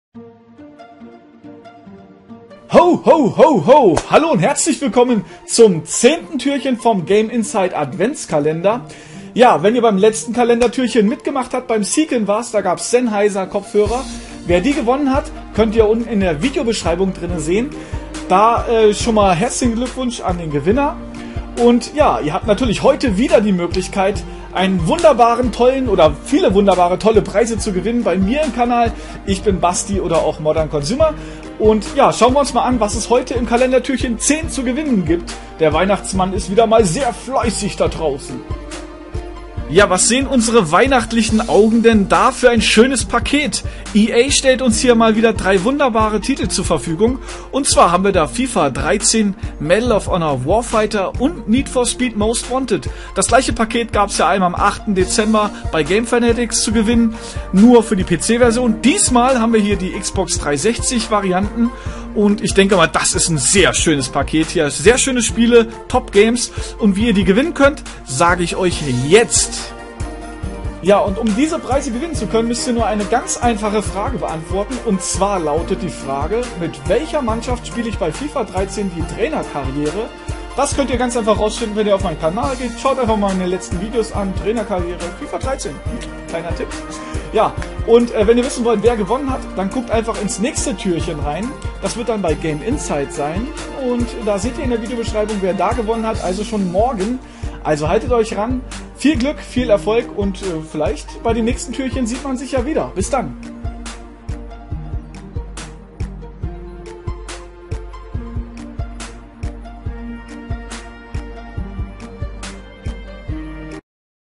Adventskalender - Türchen 10 | Gameinside Network
Adventskalender Türchen 10 hat folgendes zu bieten: Fifa 13, Need for Speed Most Wanted, Medal of Honor Warfighter - Xbox 360

Das ist der Gewinner des letzten Türchens: XxSethosxX